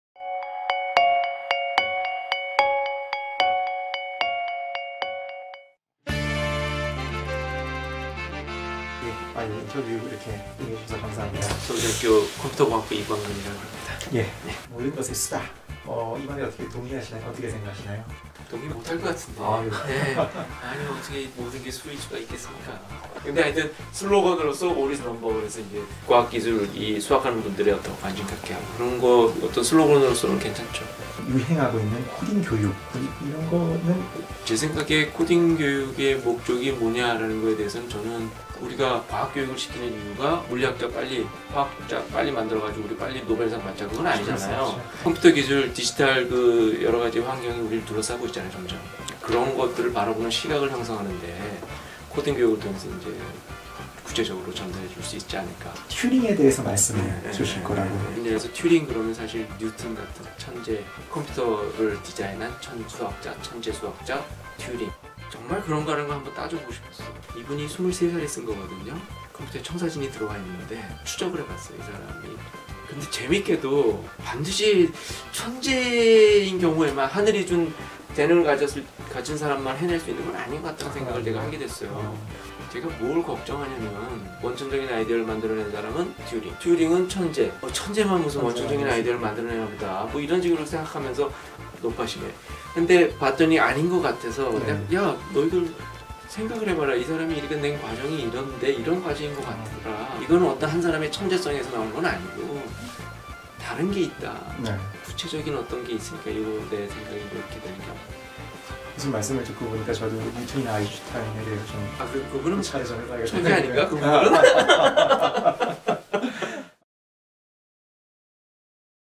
[석학인터뷰] 이광근_ 튜링은 천재가 아니다? | 2018 봄 카오스 강연 '모든 것의 수數다'
[석학인터뷰] 튜링은 천재가 아니다? | 이광근 서울대 컴퓨터공학부 교수 | 2018 봄 카오스강연 '모든 것의 수다'

석학을 만나는 시간 '카오스 석학 인터뷰'

이광근(서울대 컴퓨터공학부 교수)

■ 강연자 소개

· 現 서울대 컴퓨터공학부 교수
· 서울대 컴퓨터공학부 교수(2003-현재)
· 교육과학기술부 선도연구센터(소프트웨어 무결점 연구센터) 센터장(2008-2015)
· 과학기술부 창의연구단(프로그램분석시스템 연구단) 단장(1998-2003)
· KAIST 전산학과 교수(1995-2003)
· Bell Labs 정규연구원(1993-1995)
· 방문교수: Stanford(2017), Facebook(2016), Ecole Normale Sup´erieure Paris(2016), MIT(2012), MIT(2008), CMU(2008), Ecole Normale Sup´erieure Paris(2002)
· 상세경력 홈페이지 참고: kwangkeunyi.snu.ac.kr

휴전선 길목 산정호수 근처에서 나서 시골뜨기로 지내다 초등학교 3학년 때 서울 장위동으로 전학했다. 어린 시절 그림을 그리면 주변에서 늘 우와아아. 고등 학교 미술샘은 광근아 너 같은 애가 미대를 가야한다, 고 치켜세우셨다.
1983년 대학입학 학력고사 전국 등수가 그럴듯했다. 서울대 모든 학과에 넉넉히 합격할 태세였지만 자연대 1계열(수학, 계산학, 통계학)에 소신지원 수석입학했다. 쟨 뭔 소신이래 쑥덕이는 소리를 은근히 즐긴 구석도 있었다. 세상 통념을 희롱?
사실 컴퓨터를 전공하려거든 학부시절에는 수학이나 수학 비슷한 걸 하는 게 좋다는 이 야기를 주변에서 들었기 때문이었다. 대학입학 첫 주에 지금의 아내를 만나 씐나게 연애했다. 유학 떠나던 해 초여름 3개월 동안 우연히 책방서 만나 넝쿨처럼 울라오는 10여 권의 책들에 흠뻑 빠져 내가 뭘 해야 하는 지 다잡는 기회를 가졌다.
1995년 귀국하면서 한국에서 세계적으로 최고가 되는 전략을 잡고 악다귀해 오고 있다. 서울대와 KAIST의 학생들 덕이 크다. 좋은 나무 만지며 가구 만들기, 도장 파기, 주말 10 킬로 한강변 달리기가 취미이다.

■ 질문

Q. '모든 것은 수다' 동의하십니까?

Q. 최근 유행하는 코딩 교육에 대해 어떻게 생각하시나요?

Q. 튜링은 천재가 아니다?